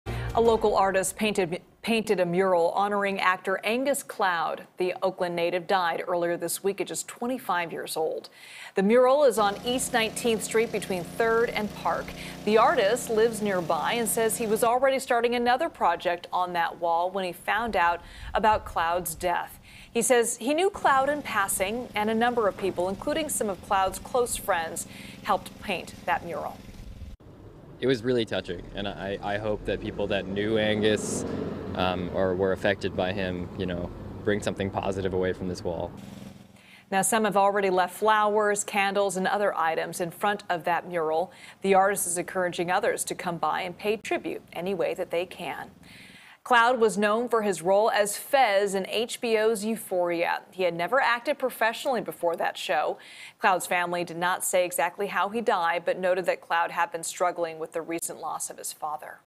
Mural in Oakland pays tribute to 'Euphoria' actor Angus Cloud
Elizabeth Cook reports on the new mural in Oakland memorializing the late 'Euphoria' actor Angus Cloud (8-2-2023)